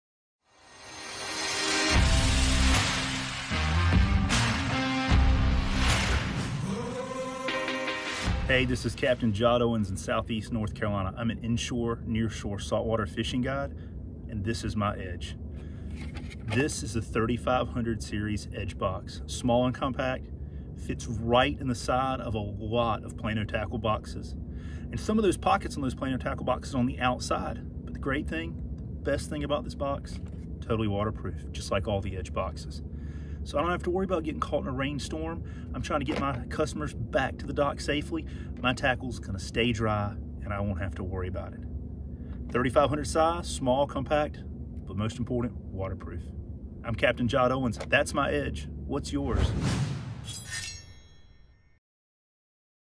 Captain Jot Owens keeping the 3500 EDGE box handy at all times! Get an EDGE with this compact box!
Anglers asked for it, and Plano delivered. Our revolutionary EDGE tackle box is now available in our smallest size – the 3500. EDGE boxes have quickly gained notoriety among fishing enthusiasts as cutting EDGE tackle storage that solves every problem on the water.